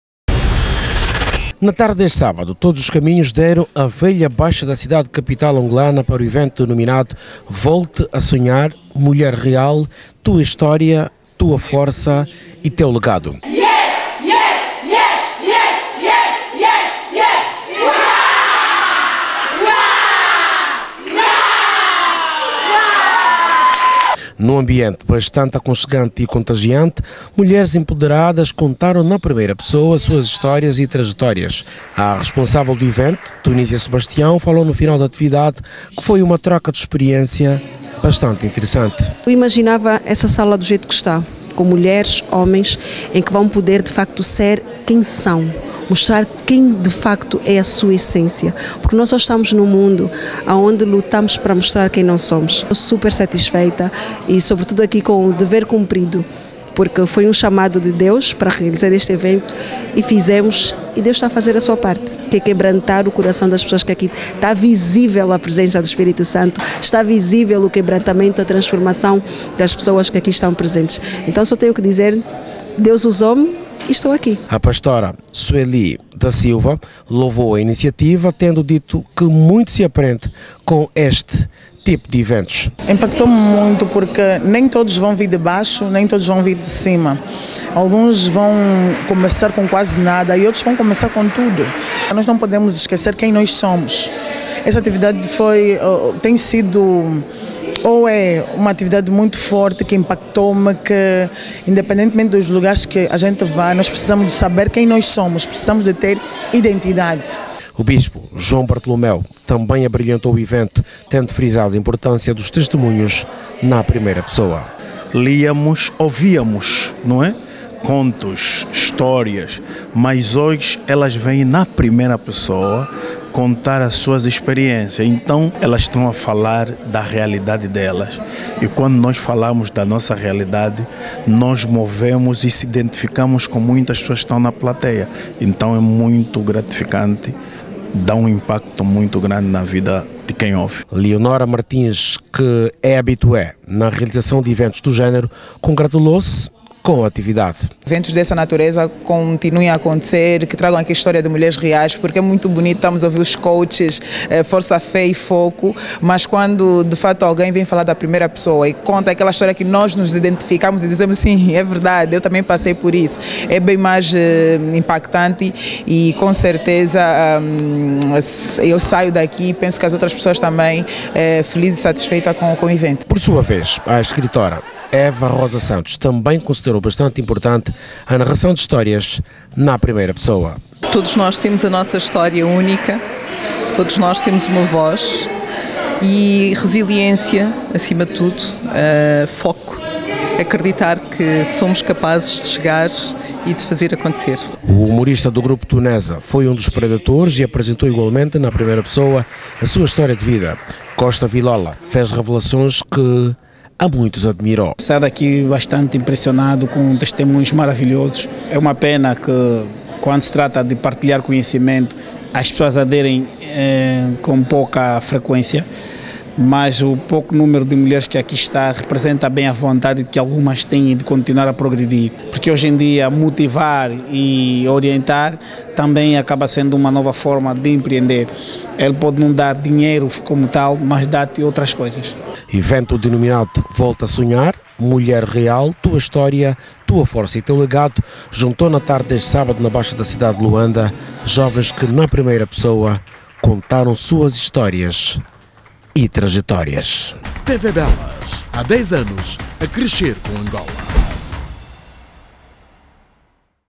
PROJECTO VOLTE A SONHAR JUNTA HISTÓRIAS E TRAJECTÓRIAS DE JOVENS DE SUCESSO
Na tarde deste sábado, todos os caminhos deram à velha baixa da cidade capital, para o evento denominado, Volte a Sonhar.
Mulher Real, tua história, tua força e teu legado.
Num ambiente bastante aconchegante e contagiante, mulheres empoderadas, contaram na primeira pessoa, suas histórias e trajectórias.
A responsável do evento, Tunísia Sebastião, falou no final da actividade, que foi uma troca de experiência bastante interessante.
Evento denominado, Volte a Sonhar.
Mulher Real, tua história, tua força e teu legado, juntou na tarde deste sábado na baixa da cidade de Luanda, jovens que na primeira pessoa, contaram suas histórias e trajectórias.